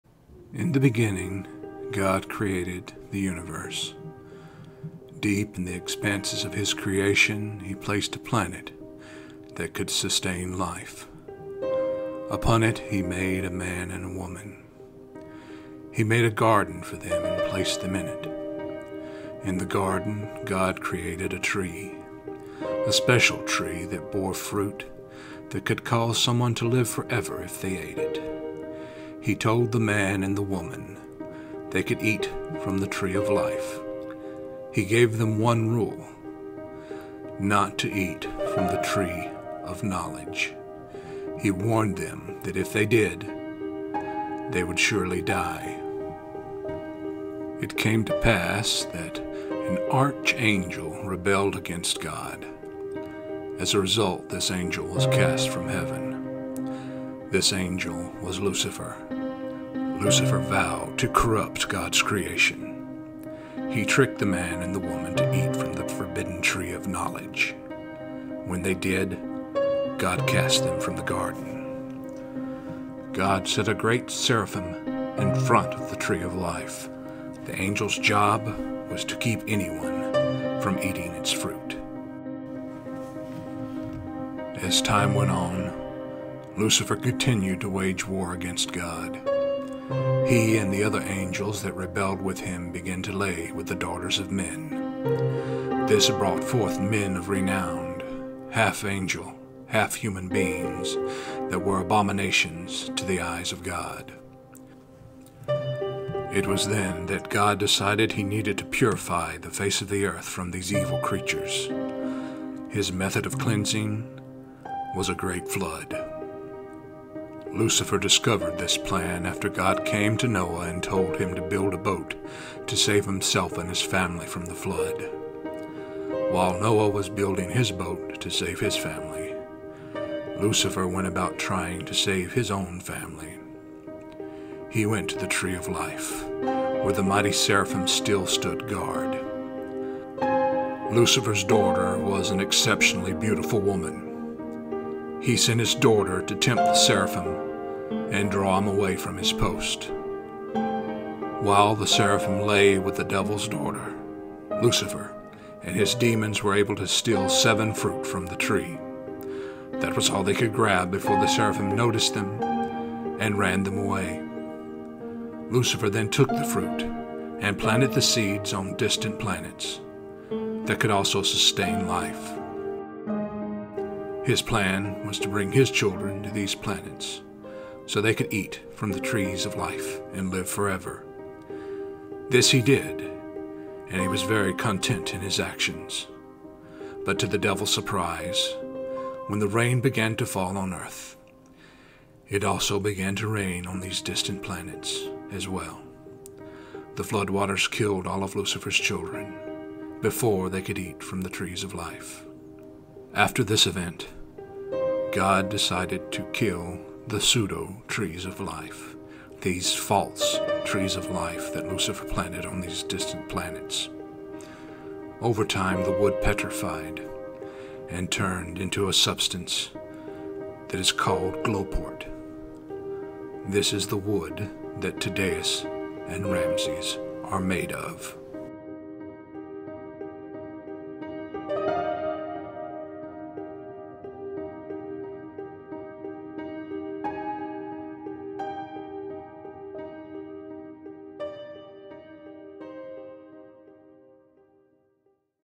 What is Gloport? What is the wood that Tudaus is made of? Where does it come from?
The Book is finally here!

Artwork made in Midjourney AI art generator.

n the beginning, God created the universe. Deep in the expanse of His creation, He placed a planet that could sustain life. Upon it, He made a man and a woman. He made a garden for them and placed them in it. In the garden, God created a tree. A special tree that bore fruit that could cause someone to live forever if they ate it. He told the man and the woman that they could eat from the tree of life.

He gave them one rule, not to eat from the tree of knowledge. He warned them that if they ate from that tree they would surly die.

It came to pass that an arch angel rebelled against God. As a result, this angel was cast from heaven. This angel was Lucifer. Lucifer vowed to corrupt God's creation. He tricked the man and the woman to eat from the forbidden tree of knowledge. When they did, God cast them from the garden. God set a great Seraphim angel in front of the tree of life . The angels job was to keep anyone from eating it's fruit.

As time went on, Lucifer continued to wage war against God. He, and the other angels that rebelled with him began to lay with the daughters of man. This brought froth men of renowned. Half angel, half human beings that were an abomination to the eye's of God. It was then that God decided that He needed to purify the face of the earth from these evil creatures. His method of cleansing was a great flood.

Lucifer discovered this plan after God came to Noah and told him to build a boat to save himself and his family from the flood. While Noah was building his boat to save his family, Lucifer went about trying to save his family. He went to the tree of life were the mighty Seraphim still stood guard. Lucifer's daughter was and exceptionally beautiful woman. He sent his daughter to temp the Seraphim and draw him away from his post.

While the Seraphim lay with the devils daughter, Lucifer and his demons were able to steal 7 fruit from the tree. That was all they could grab before the Seraphim noticed them and ran them off. Lucifer then took the fruit and planted the seeds on distant planets that could also sustain life.

His plan was to bring his children to these planets so they could eat from the trees of life and live forever. This he did, and he was very content in his actions. But to the devils surprise, when the rain began to fall on earth, it also began to rain on these distant planets as well. The flood waters killed all of Lucifer's children before they could eat from the trees of life.

After this event, God decided to kill these the sudo trees of life. These false trees of life that Lucifer planted on these distant planets. Overtime, the wood petrified and turned into a substance that is called gloport.  This is the wood that Tudaus and Ramzies are made of.